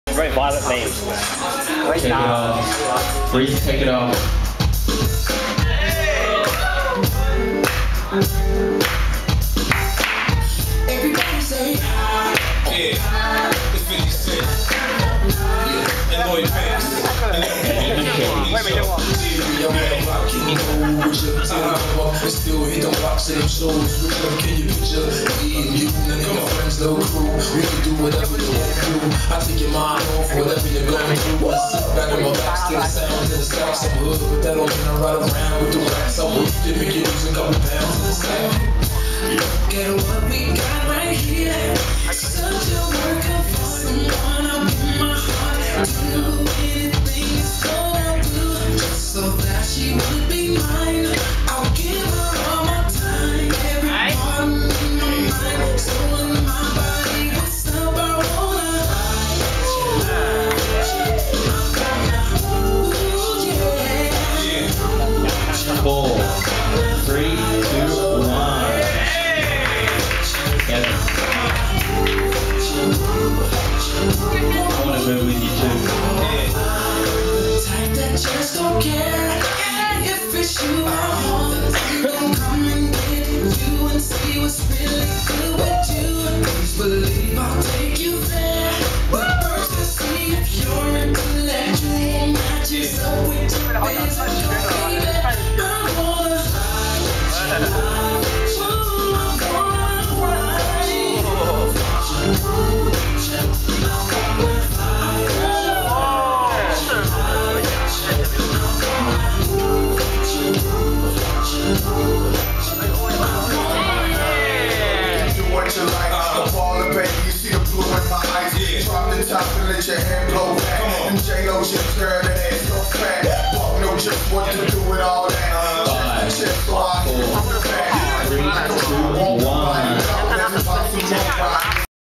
Eddy vs Kevin - Prelims - Charlene's Funktion
Charlene's Funktion (Sat 4th June 2017) at 80 Proof.
Prelims - Eddy vs Kevin

Judge: Charlene
DJ: Rhyspect